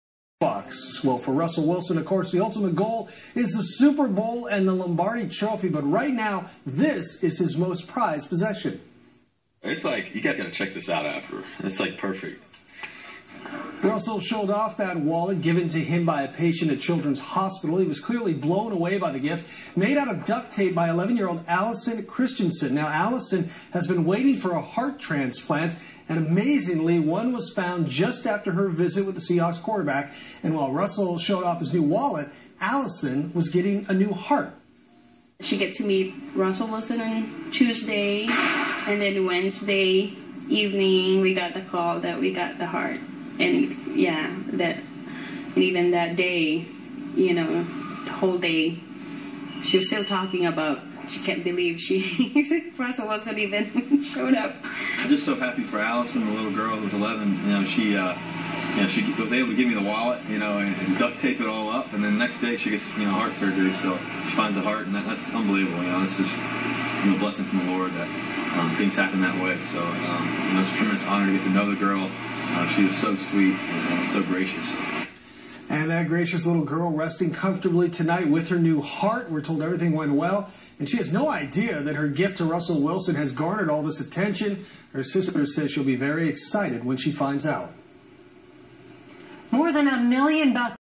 Russell Wilson's Duct Tape Wallet
Sorry for the quality. Was taken with my phone off of my TV screen. The story is about a little girl in Seattle Children's Hospital waiting on a Heart. She was able to meet Russell and give him a wallet that she made for him. The next day a heart was found for the little girl. Major respect to Russell, Seattle Children's and that strong little girl!